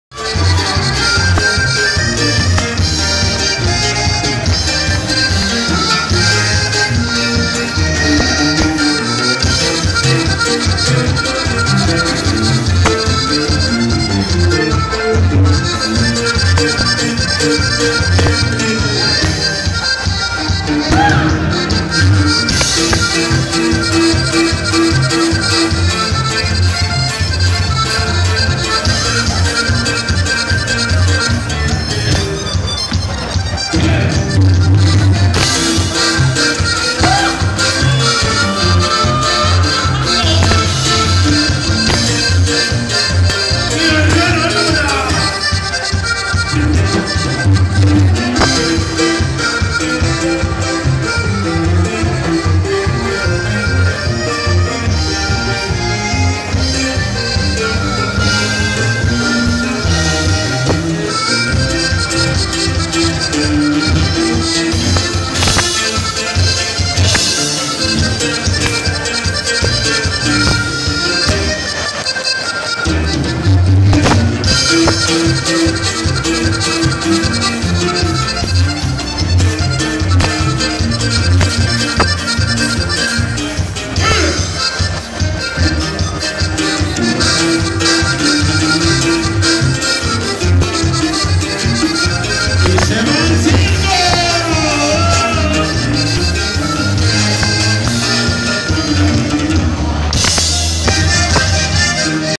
Baile de enfermería universitario
Muy lindo y divertido baile gracias a los directivos por reconocer la gran labor de enfermería 🥰🥳